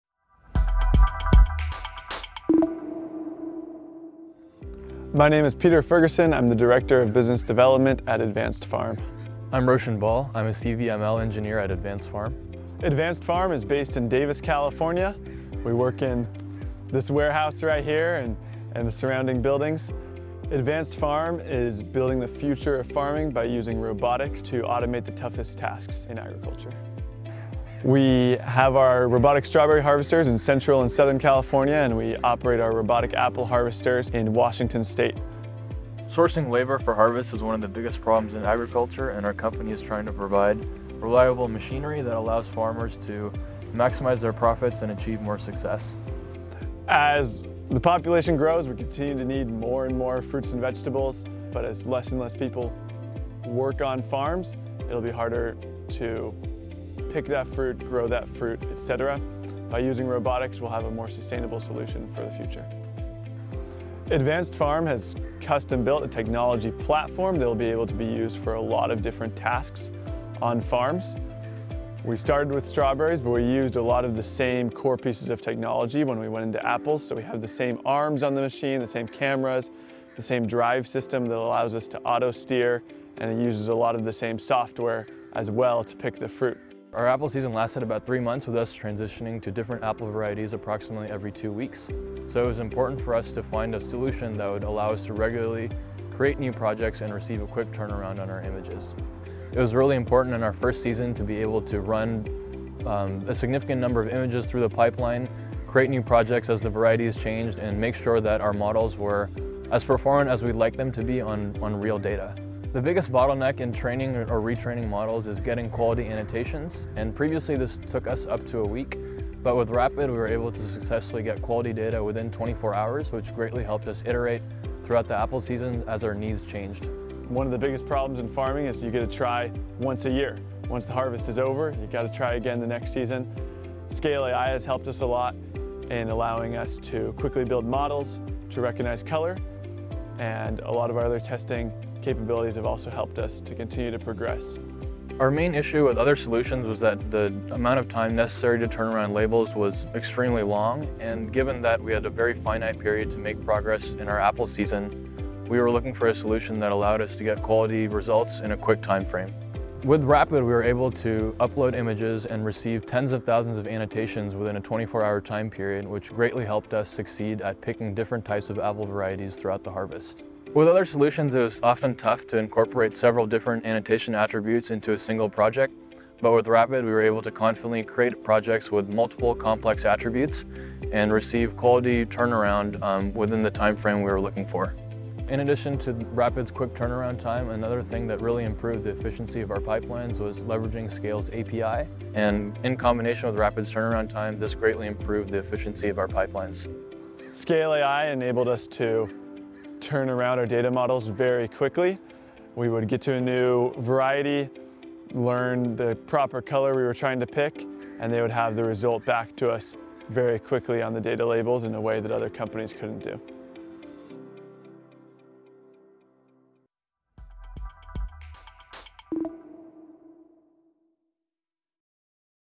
Customer Success Story: advanced.farm | Scale AI
Advanced.farm is building the future of farming by using robotics to automate the toughest tasks in agriculture, making farms safer and more productive. Focusing on the most resource-intensive tasks, advanced.farm has automated solutions for strawberry and apple picking and will be scaling to other challenges in the near future. Advanced.farm is building the advanced platform for agricultural equipment by combining advanced robotics, autonomous navigation, and a state-of-the-art computer vision machine learning (CVML) stack